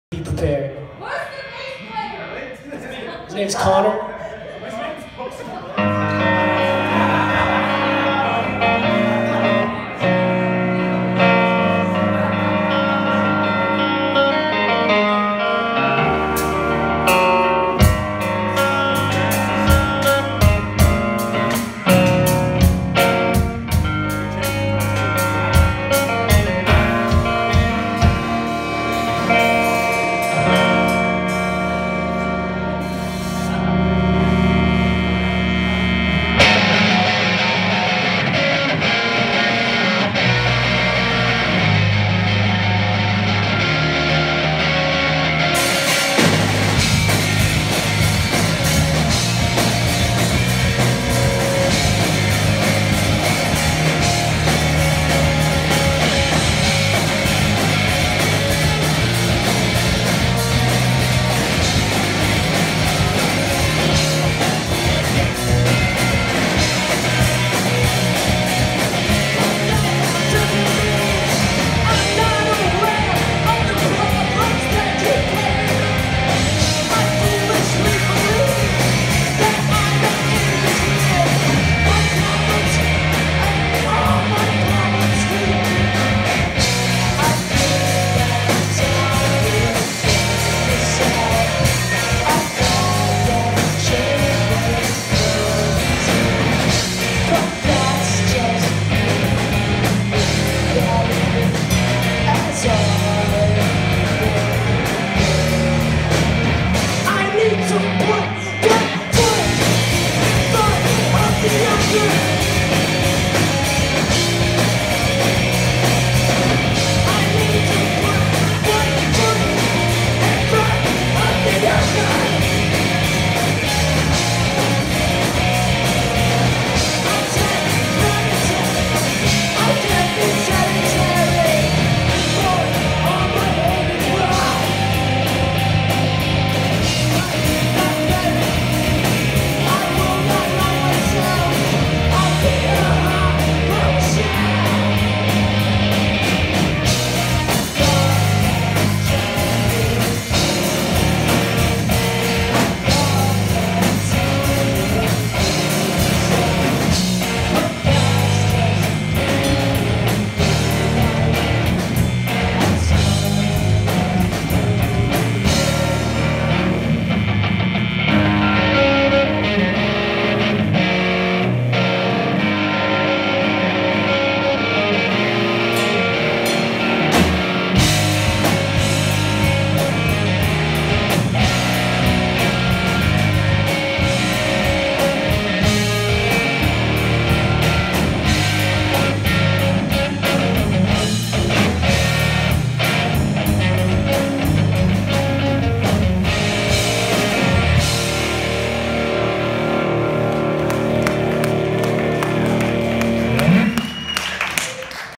Pretty Shanks Live
In San Francisco.
Connor Richardson (Bass), Devin Dohrmann (Drums), Vincent Tinsay ( Vocals/Guitar)